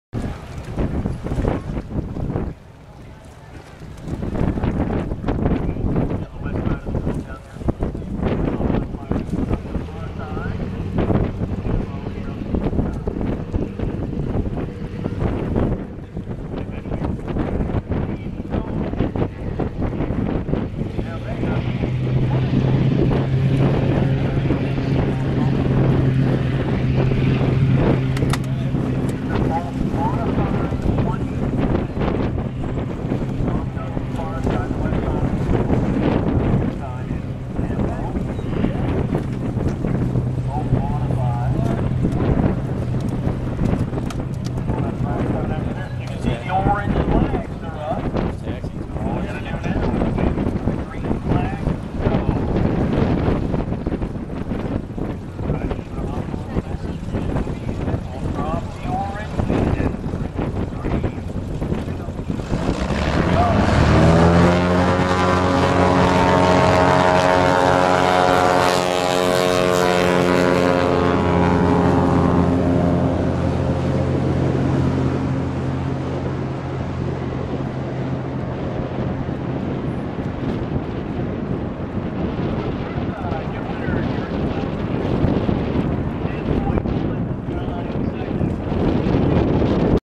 Cessna 180 Versus Cessna 180 at the Greenville Fly-In
One of the takeoff competitions between two Cessna 180's at the Greenville Seaplane Fly-In in 2010. The 180 with the upgraded engine actually loses to the traditionally O-470 powered plane.